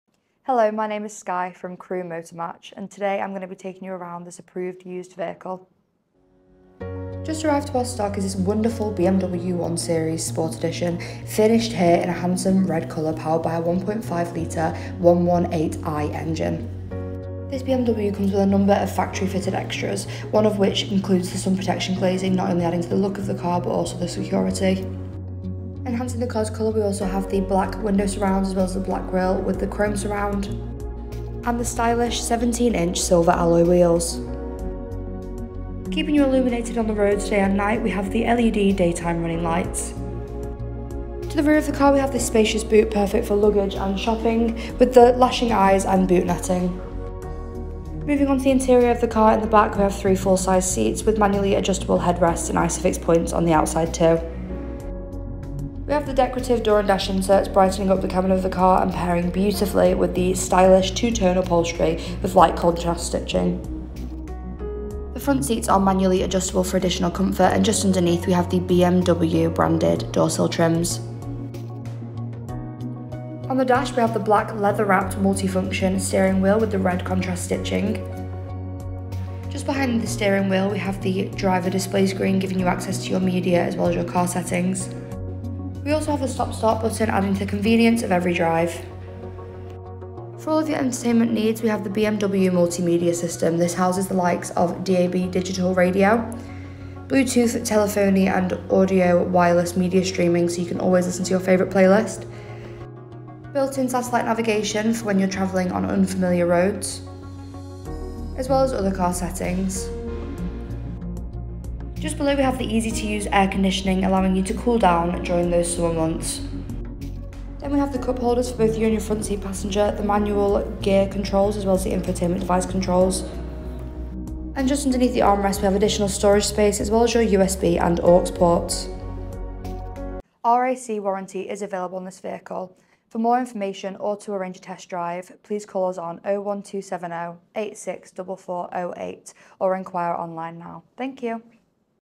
Approved Used BMW 1 Series 1.5 118i Sport YH65VKJ | Motor Match Crewe
This BMW has just come into stock at Motor Match Crewe.

Upgraded extras:
Driver Comfort Package
Enhanced Telephone Preparation with USB Audio Interface
Sun Protection Glazing